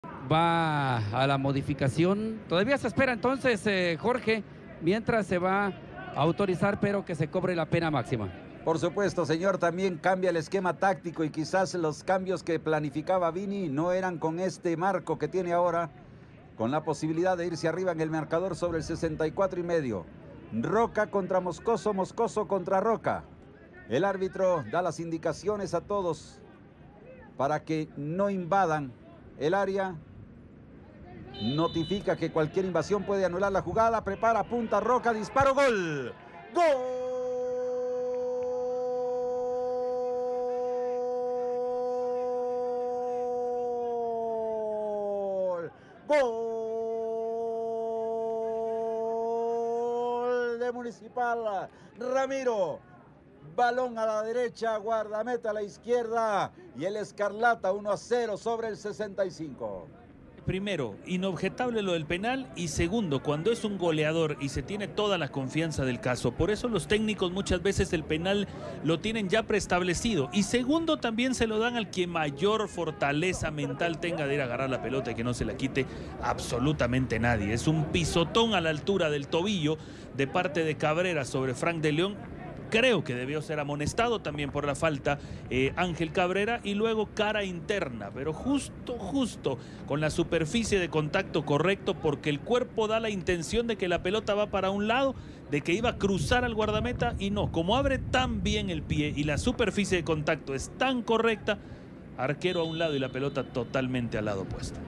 Gol de Ramiro Rocca, Municipal 1-0 Cobán Imperial
Gol para Municipal, Anotación de Ramiro Rocca para Municipal el 1-0 ante Cobán Imperial al minuto 64.